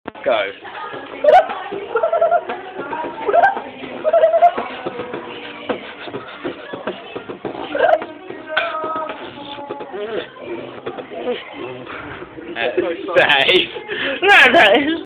Beat boxing wonderboy
tis loud cos of very rude people, but here is mick esquire whipping out some cheeky beats